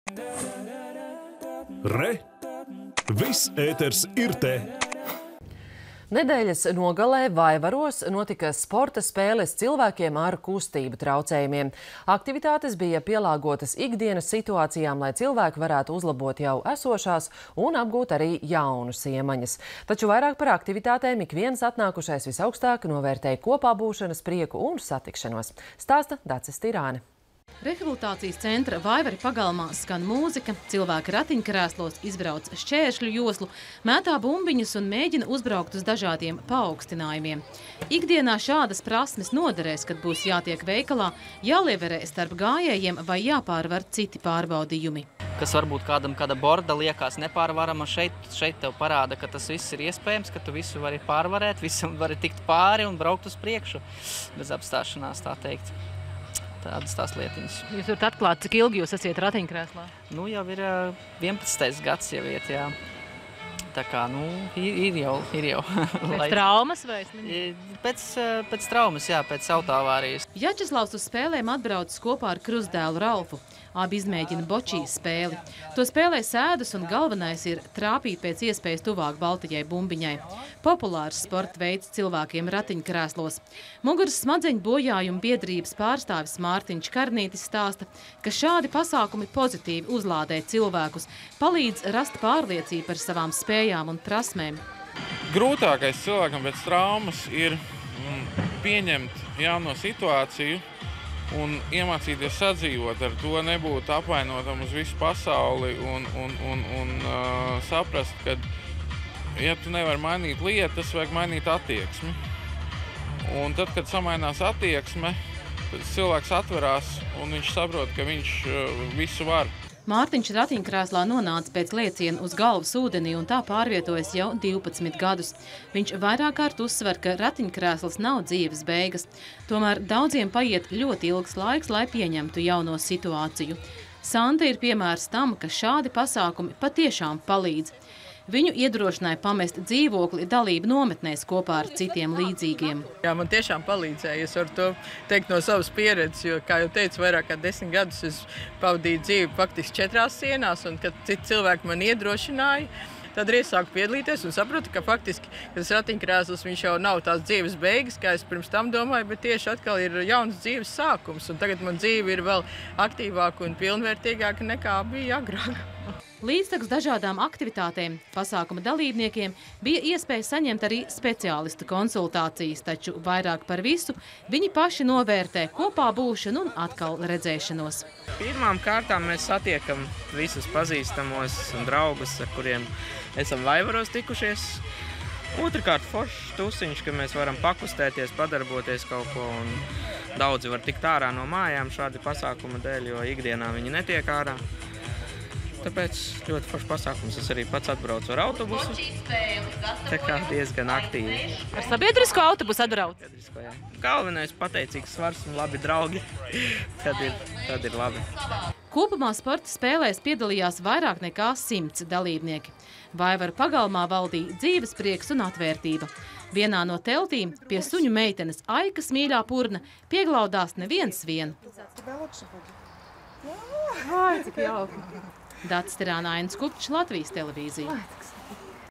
Cilvēki ar kustību traucējumiem apgūst jaunas iemaņas.
Nedēļas nogalē Vaivaros notika sporta spēles cilvēkiem ar kustību traucējumiem. Aktivitātes bija pielāgotas ikdienas situācijām, lai cilvēki varētu uzlabot jau esošās un apgūt jaunas iemaņas. Taču vairāk par aktivitātēm, ikviens atnākušais visaugstāk novērtēja kopā būšanas prieku un satikšanos. Stāsta Dace Stirāne .